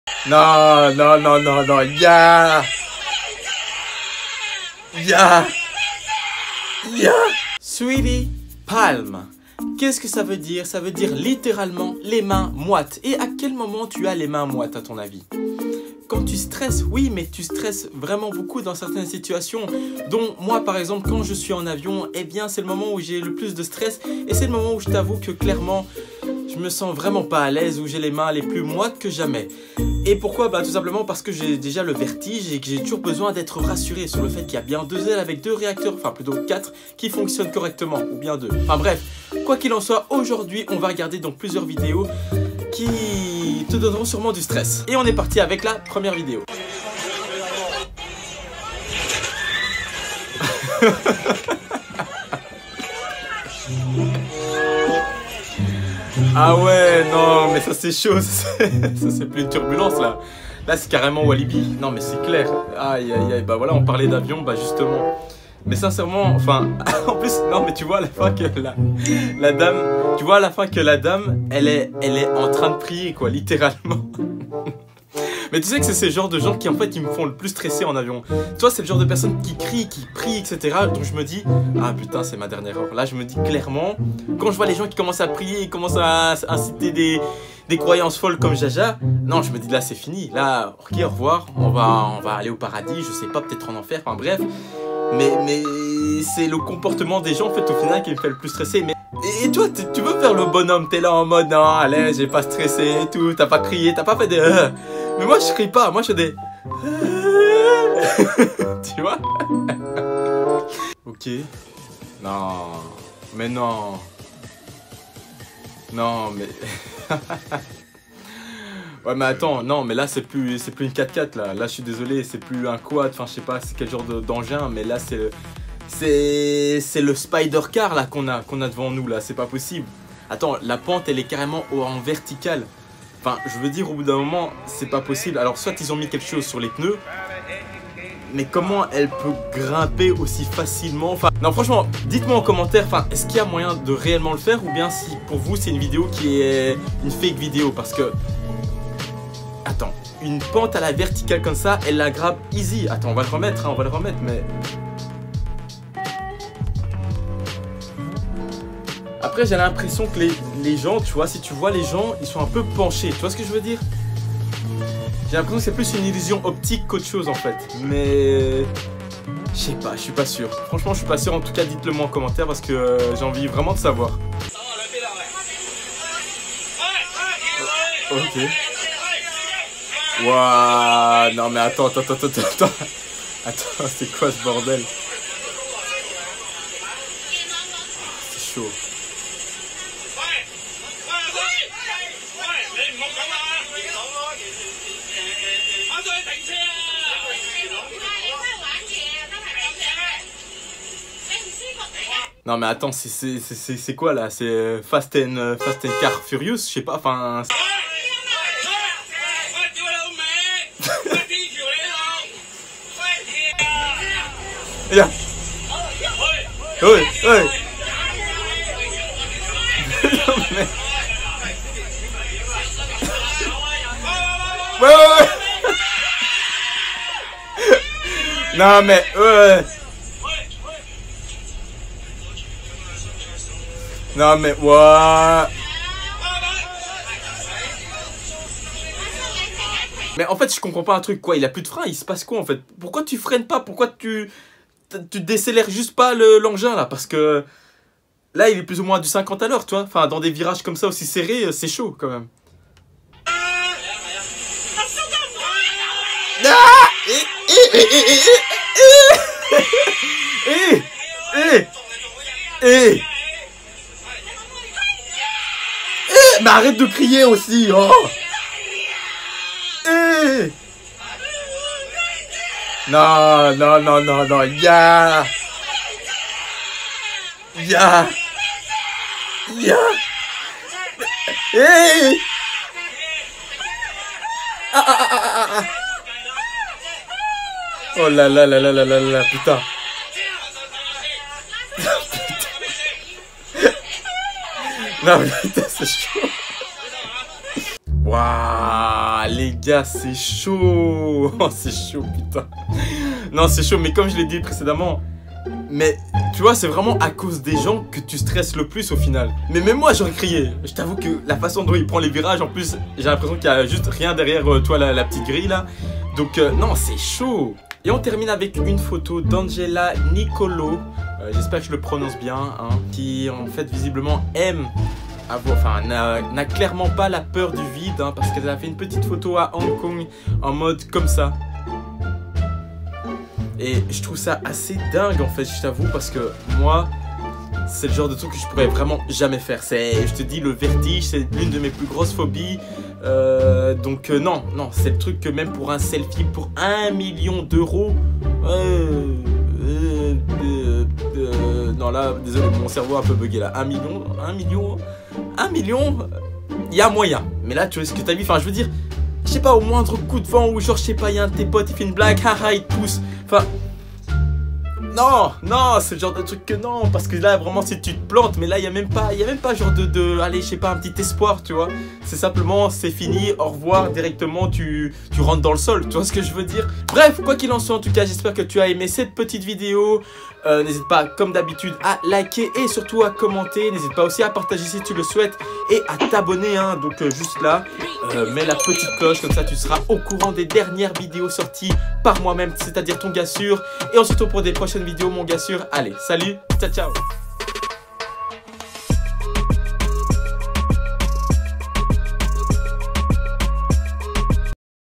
POURQUOI ILS FONT CA ? 😱
Salut tout le monde , qui n'as jamais eu peur de tomber ou bien de mourir à cause d'une situation ? Aujourd'hui on va voir mes grosses frayeurs, des situations ou j'aimerais à aucun moment être à leurs place 😛

Bienvenue dans la ChillaBang #8 from ton gars sur, Red Tylerr

J'espère que tu vas bien et bienvenue chez toi, pose toi, met toi bien, les commentaires sont là si tu veux t'exprimer :P